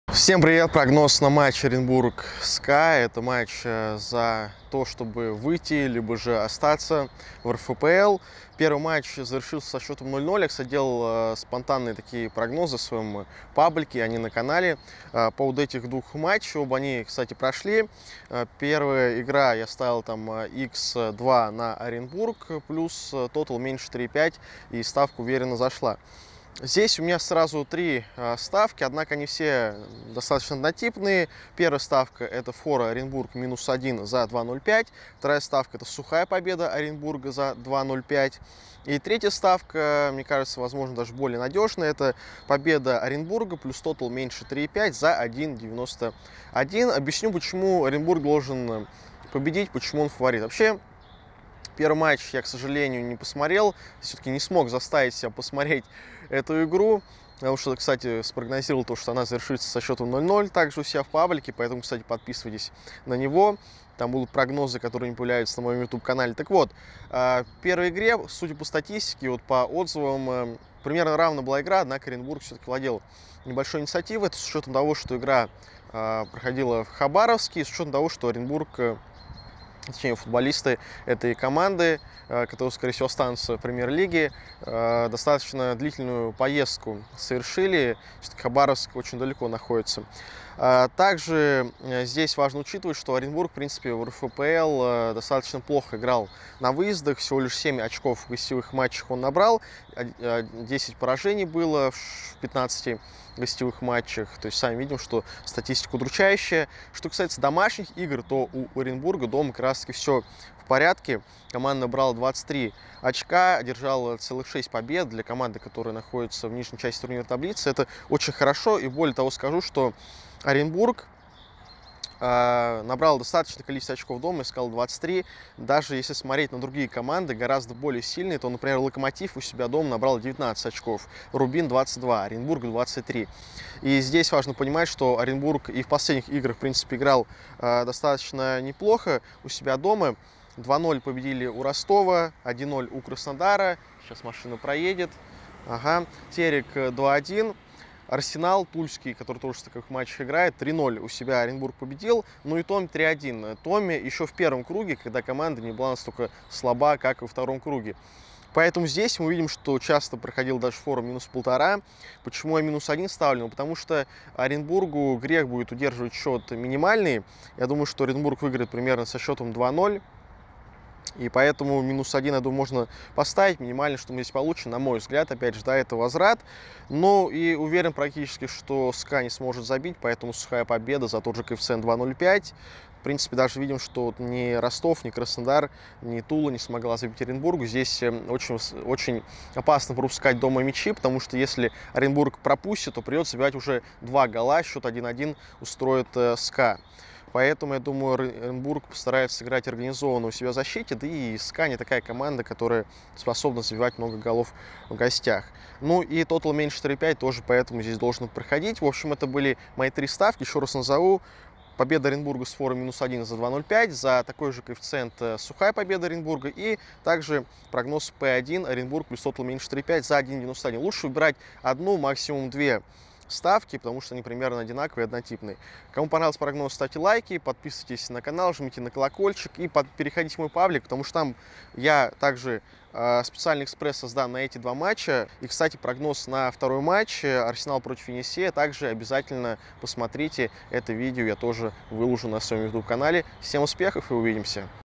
Оренбург-СКА. Прогнозы и ставки. Кэф. 2,05
Прогнозы и ставки на матч Плей-офф за право играть в Премьер-Лиге: Оренбург - СКА-Хабаровск!
Полный анализ!